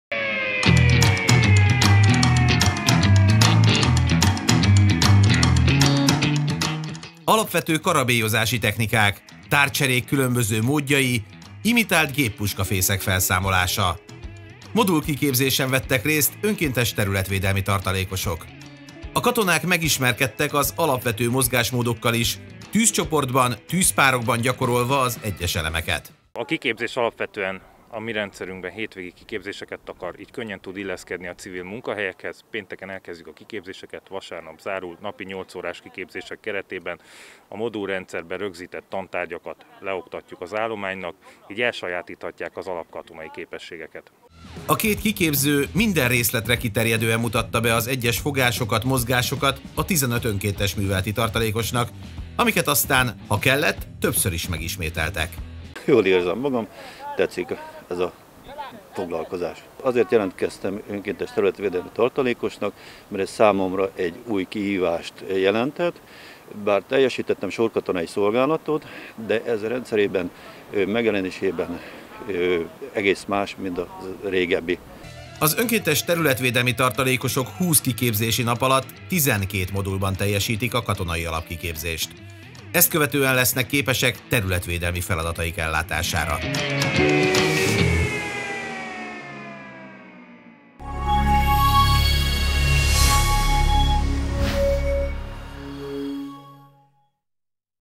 Kiképzésen a területvédelmi tartalékosok
A terepen történő alapvető mozgásmódokat sajátítottak el önkéntes területvédelmi tartalékosok Szekszárdon, február 27-én, szombaton.